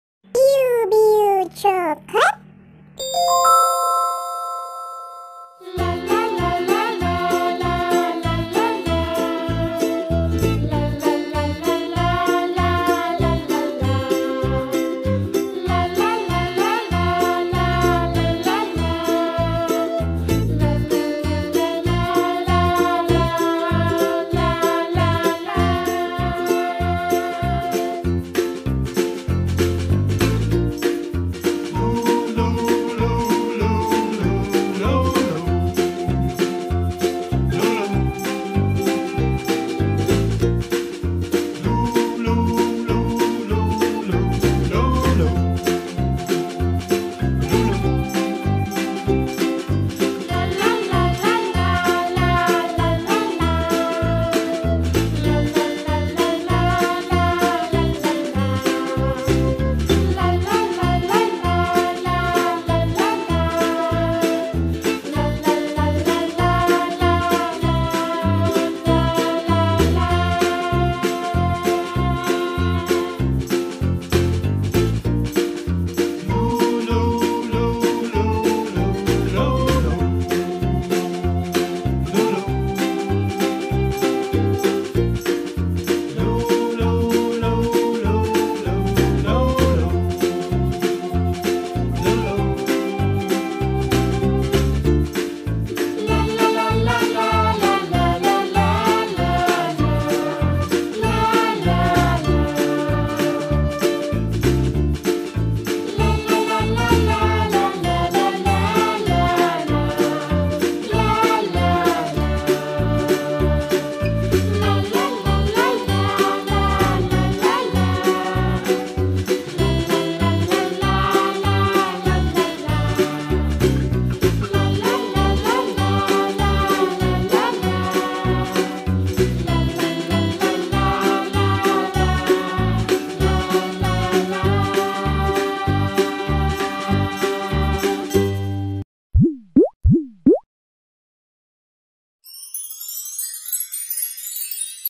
Pups Make a Surprise on Birthday Party - Paw Patrol fanmade coloring story
by: BiuBiu Chocolate | Paw Patrol Drawing Tutorial
Paw Patrol Episodes :